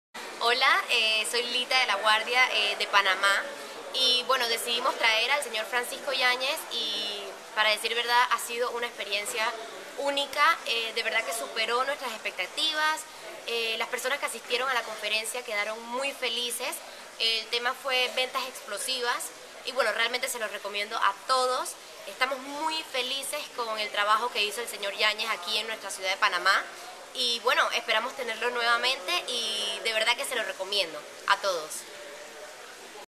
Francisco Yañez Testimonial Panama
Francisco Yáñez Testimonio curso de ventas explosivas 4 de Octubre en Panama